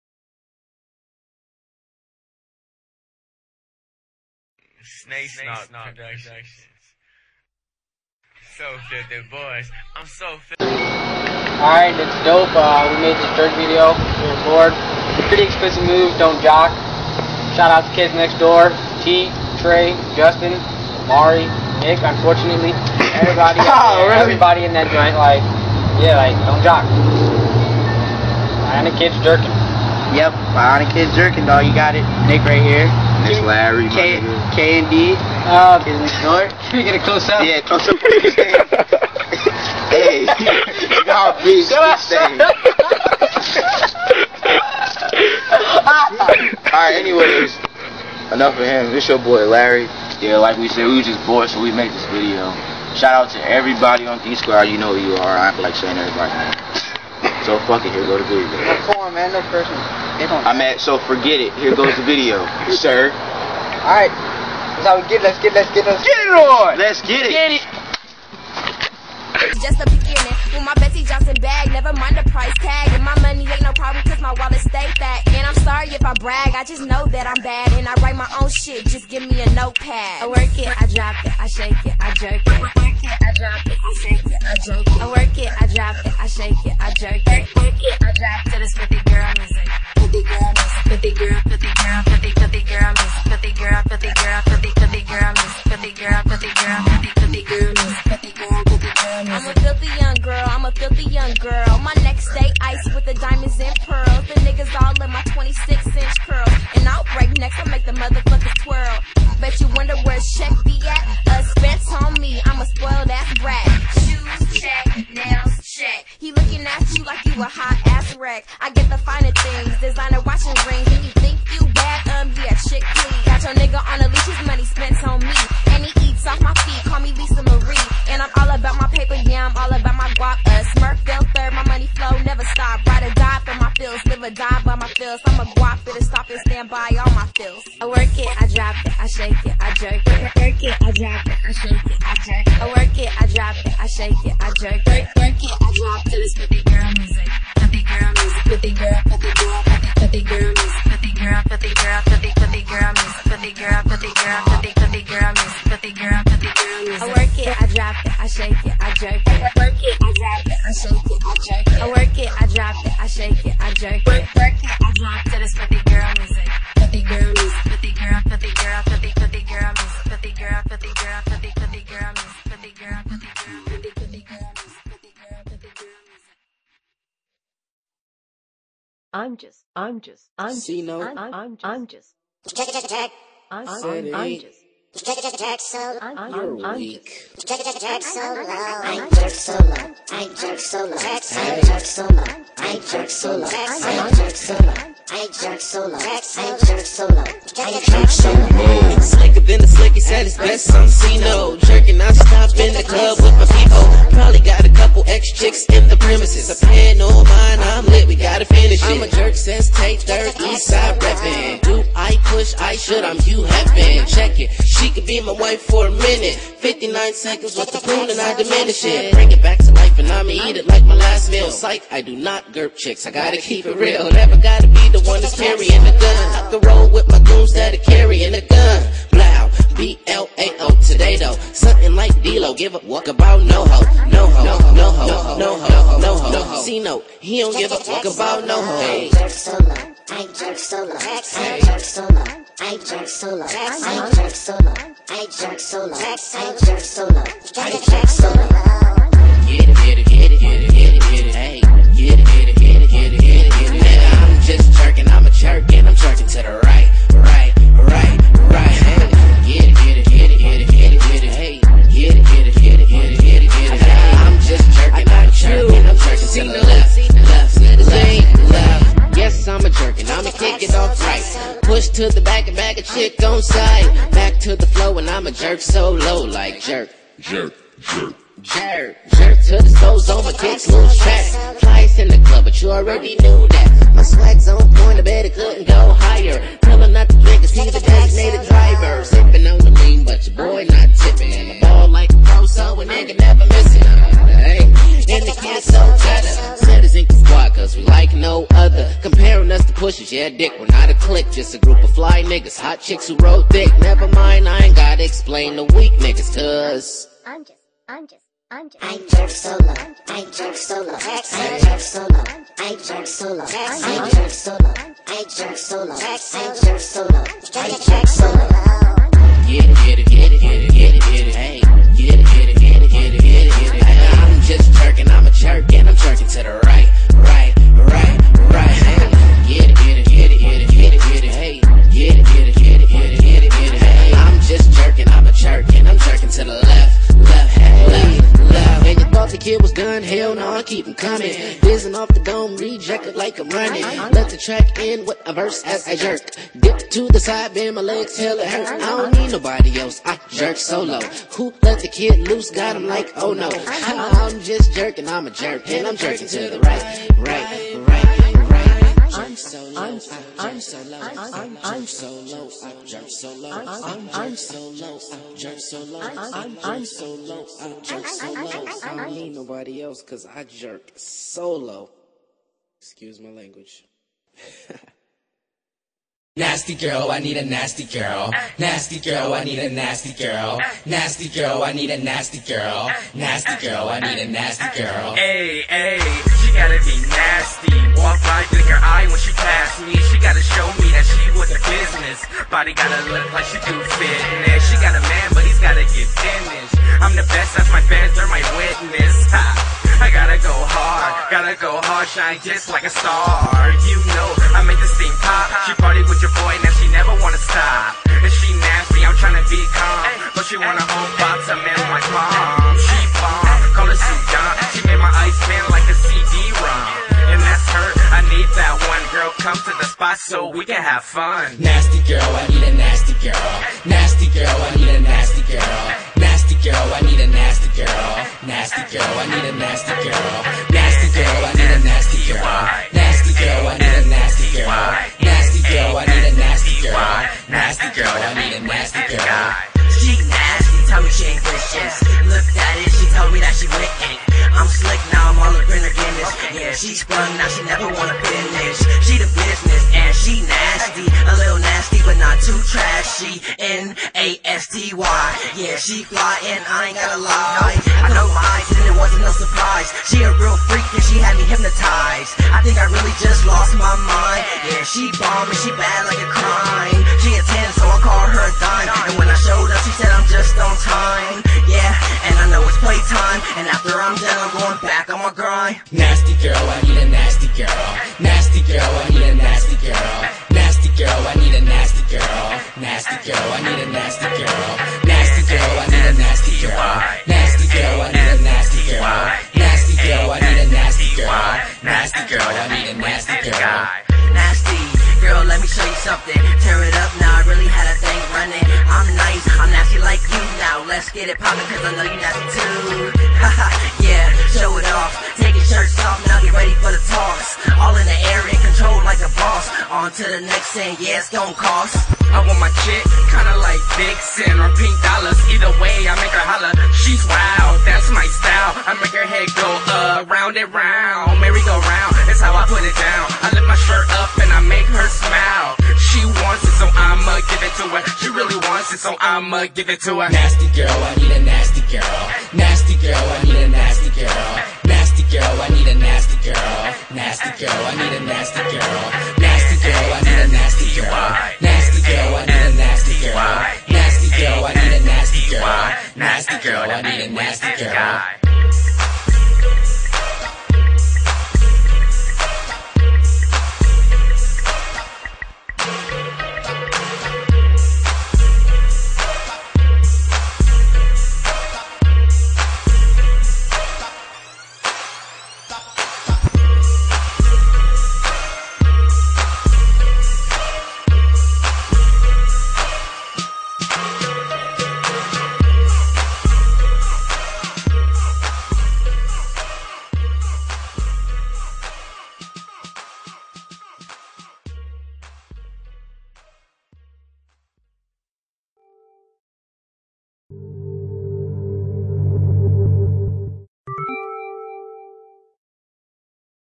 jerkin on a boring day
Nick A.K.A Heavyweight da jerk(KnD), sean A.K.A Dope da jerk(KnD), jay A.K.A Spectacular(BionicKidz), and Deshawn(GeekSquad)